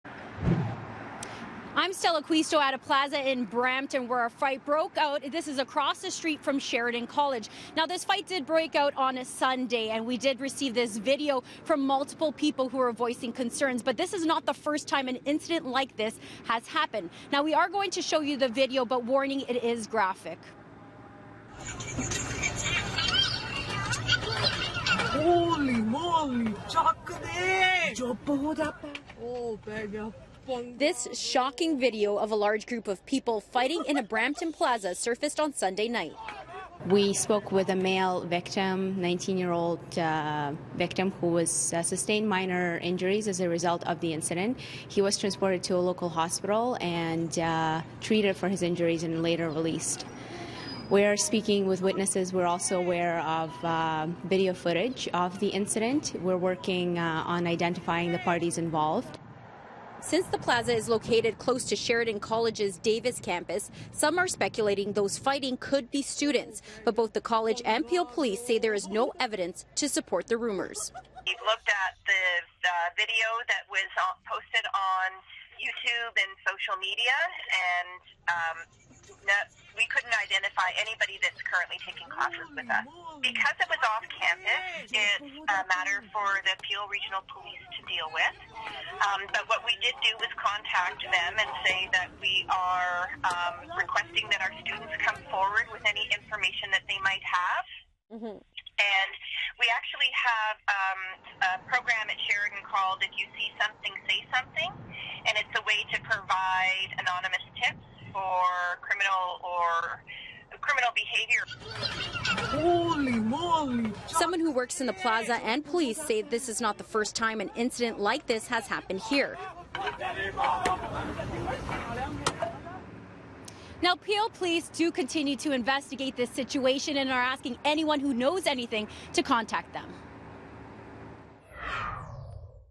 Brampton plaza brawl *Warning: graphic content*
A violent brawl at a Brampton plaza has prompted Peel police to launch an investigation. Warning: some viewers may find the footage disturbing.